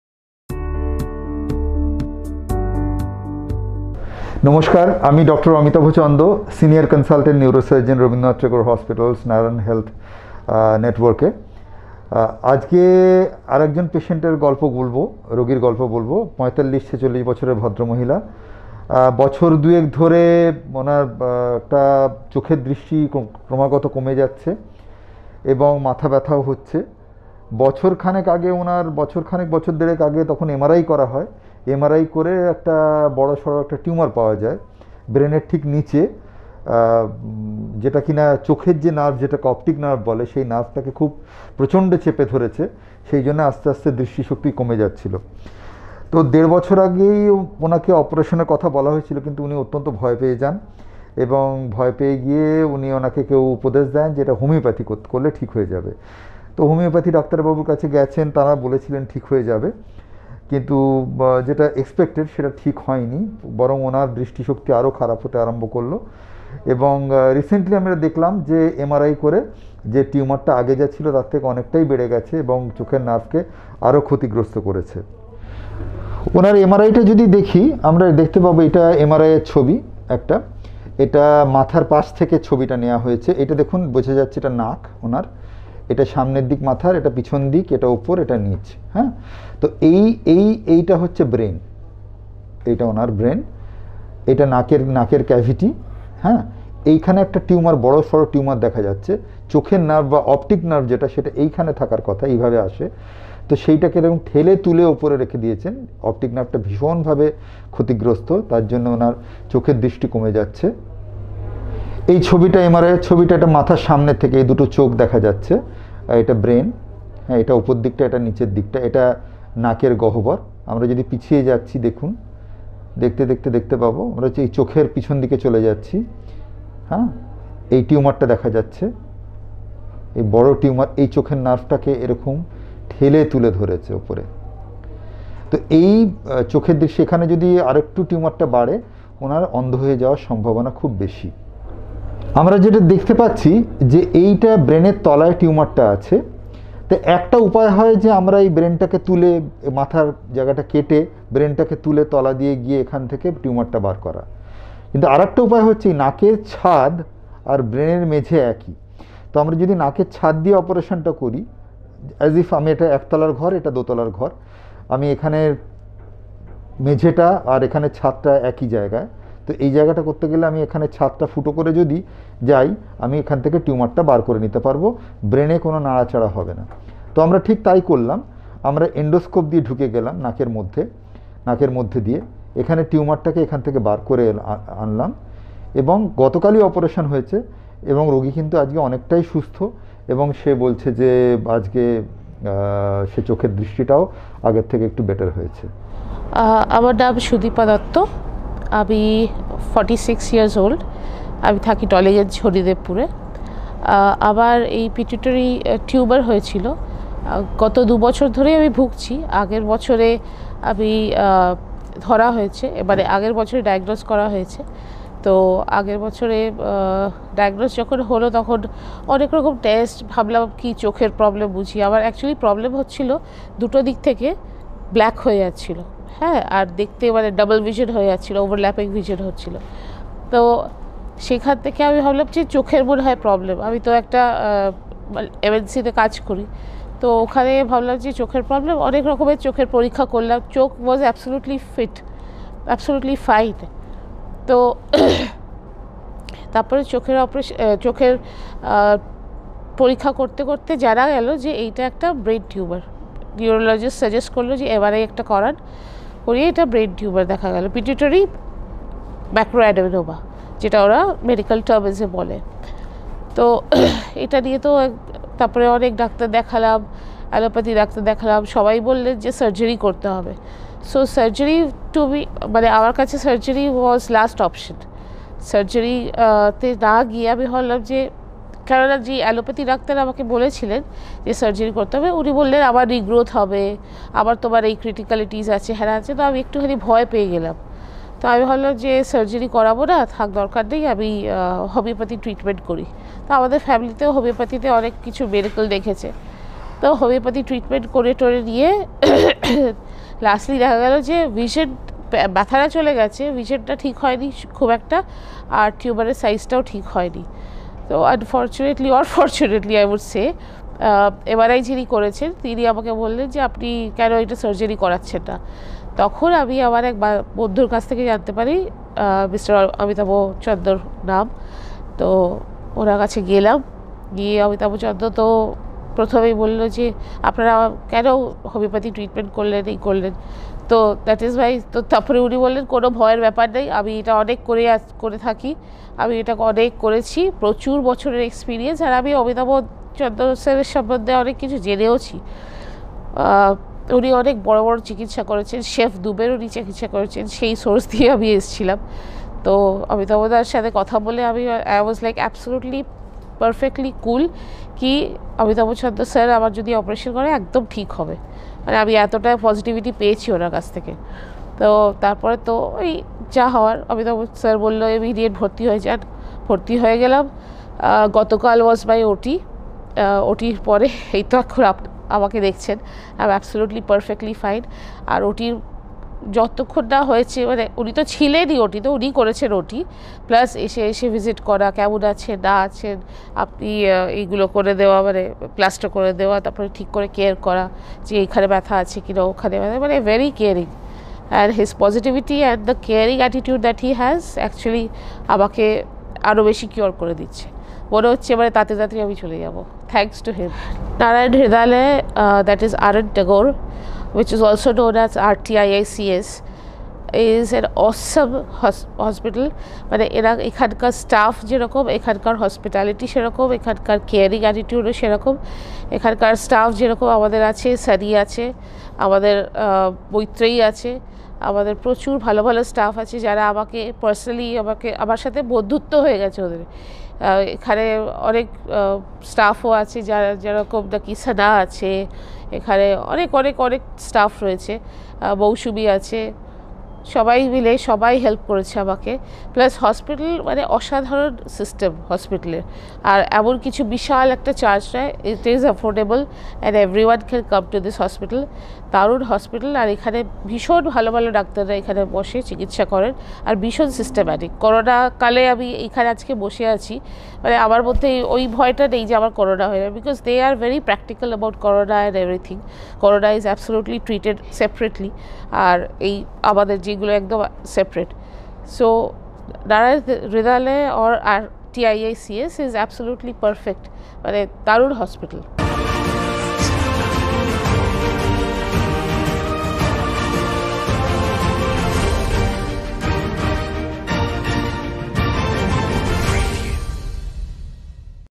Patient shares her experience after Endoscopic Pituitary Tumor resection by Dr. Amitabha Chanda
This lady presented with progressive dimness of vision. She was diagnosed with a large pituitary tumor. She underwent endoscopic pituitary tumor resection. She had an excellent recovery.